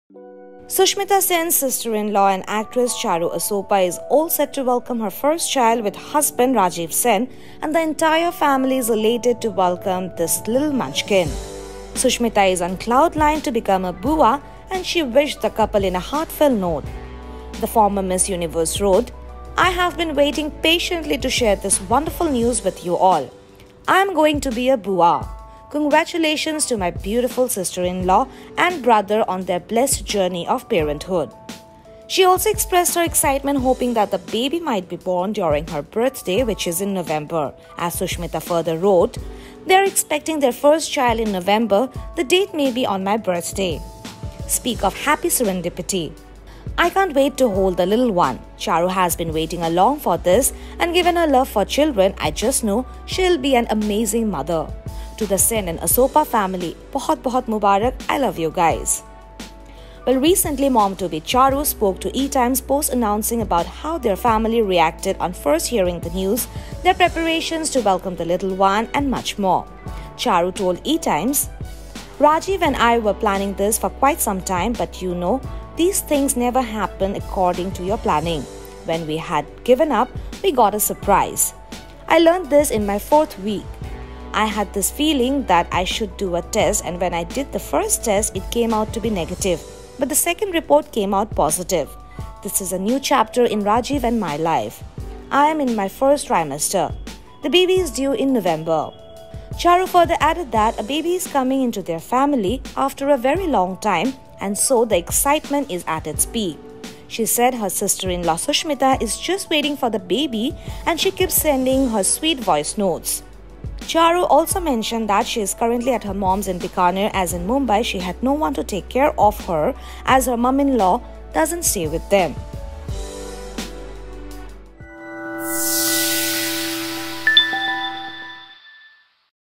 Sushmita Sen reacts to Charu Asopa's pregnancy, says 'can't wait to hold the little one'
Sushmita Sen's sister-in-law and actress Charu Asopa is all set to welcome her first child with husband Rajeev Sen and the entire family is elated to welcome this little munchkin. Sushmita is on cloud nine to become a 'bua' and she wished the couple in a heartfelt note.

sushmitasen